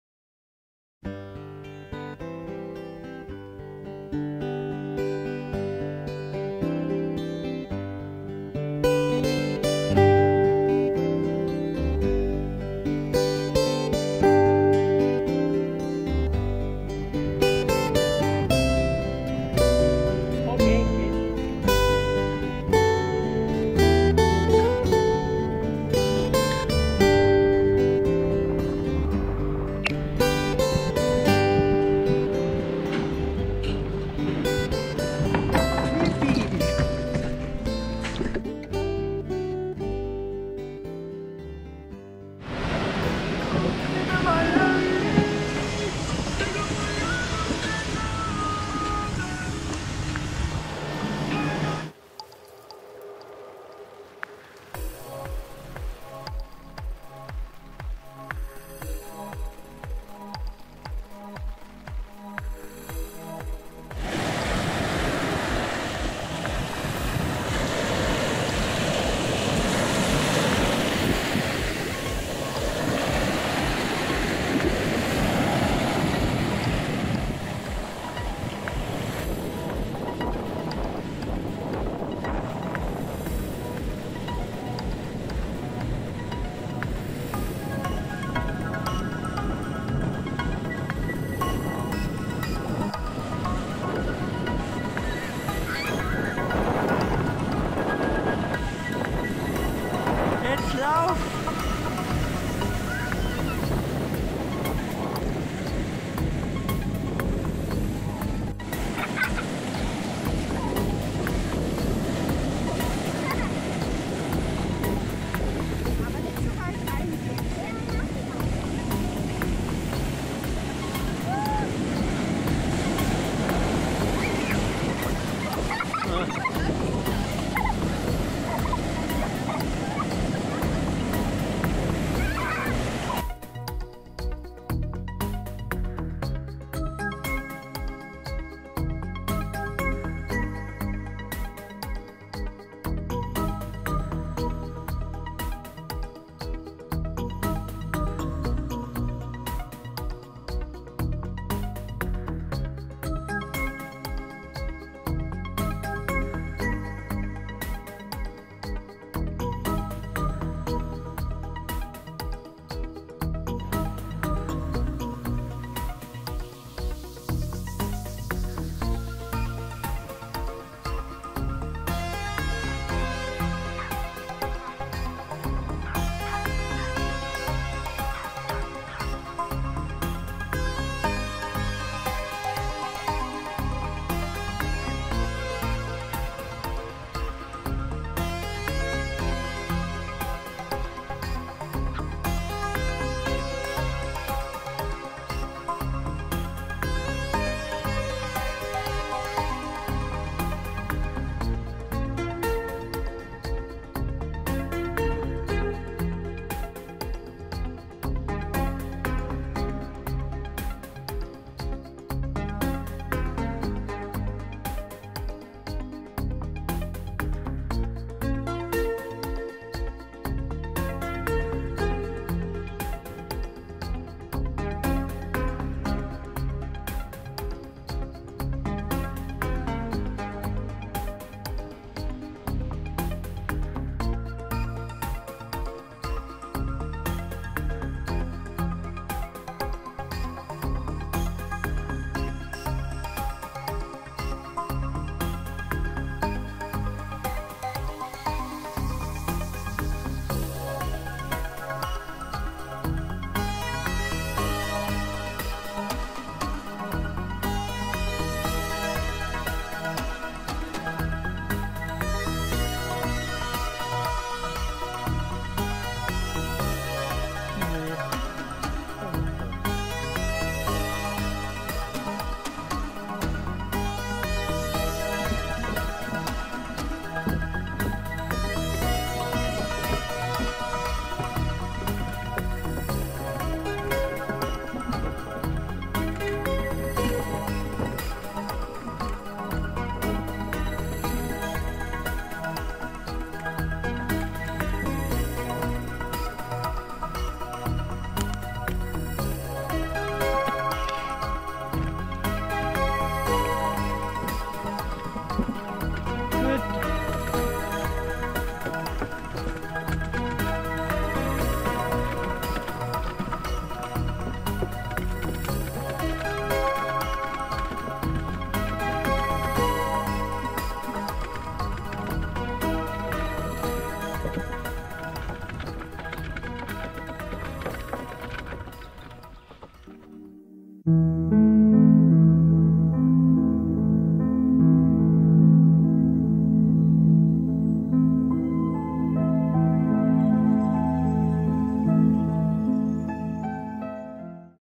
Urlaub Sotogrande 2024
Sotogrande an der analusischen Küste gehört zu den schönsten Regionen in Spanien. Der luxuriöse Ort ist nicht nur sicher, was uns beonsders gefallen hat: es wird dort sehr viel Wert auf auf die Natur und ihren Schutz gelegt.
Die Strände sind traumhaft. dies besonders in der Vorsaison wo wir da waren.